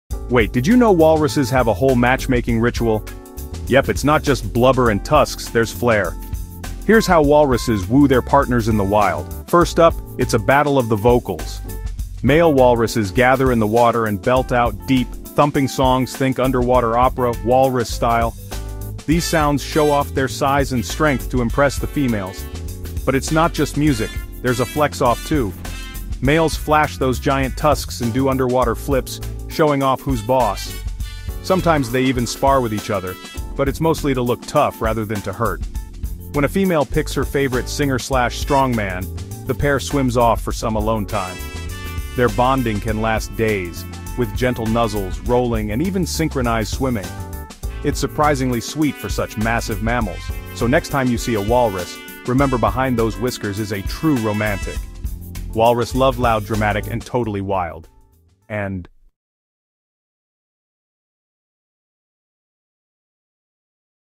Love in the Arctic: How Walruses Woo and Find Their Partners
In the icy expanse of the Arctic, love is as wild and unpredictable as the environment itself. "Love in the Arctic" takes you on a fascinating journey into the intimate world of walruses, where courting rituals, social dynamics, and survival instincts intersect. From vocal displays and elaborate physical gestures to the unique ways they choose their mates, walruses have a surprisingly complex love life. This exploration dives deep into the behaviors, challenges, and bonds that define walrus relationships, offering a rare glimpse into the raw beauty of Arctic romance. Discover how these magnificent creatures navigate love in one of the harshest environments on Earth.